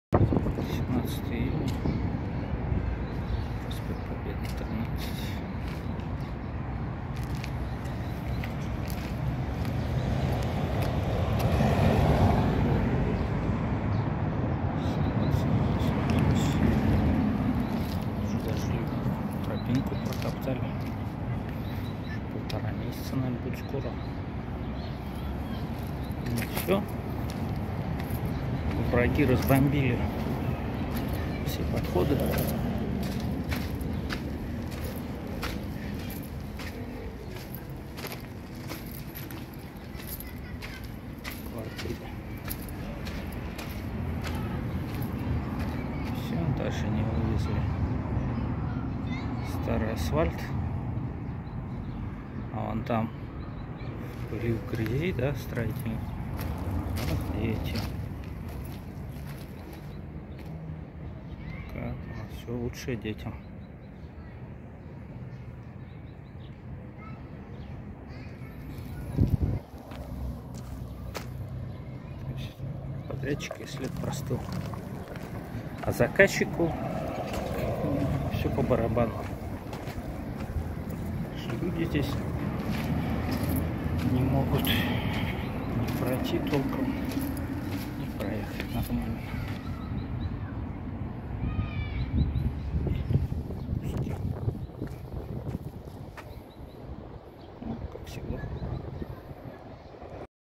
ДУРАКИ И БЛАГОУСТРОЙСТВО В "ГОРодЕ ДЛЯ ЛЮДЕЙ" 17.06.22 БОРИС БОДИЕВ
БОРИС БОДИЕВ
17.06.22
ДУРАКИ И БЛАГОУСТРОЙСТВО ДВОРОВ НА ПРОСПЕКТЕ ПОБЕДЫ,13 В "ГОРодЕ ДЛЯ ЛЮДЕЙ"
(Больше месяца ни пройти, ни проехать по центральной, видовой улице )